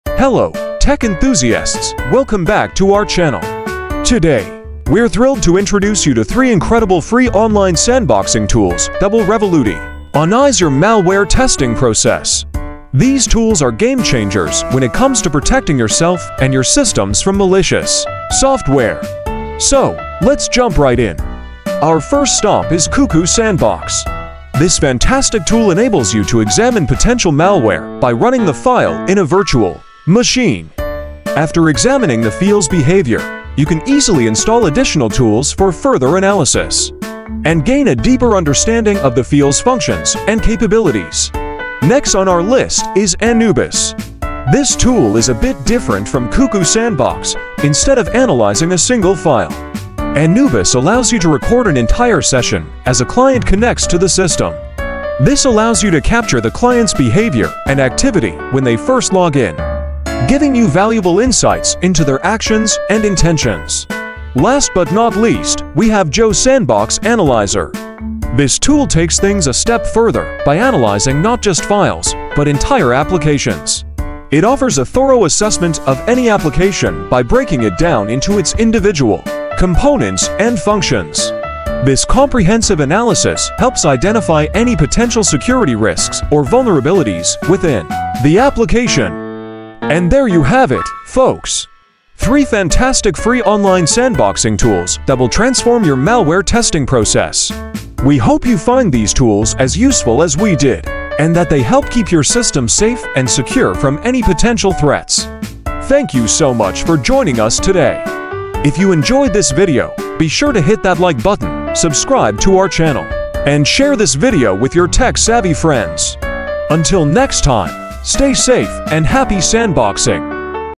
Discovering Free Online Sandboxing Tools for Application Testing!
In this video, we delve into the world of free online sandboxing tools that are essential for testing applications. Whether you're a developer, QA engineer, or simply curious about software testing, these tools offer a convenient and efficient way to test applications in a secure environment. Join us as we explore the best options available for application testing!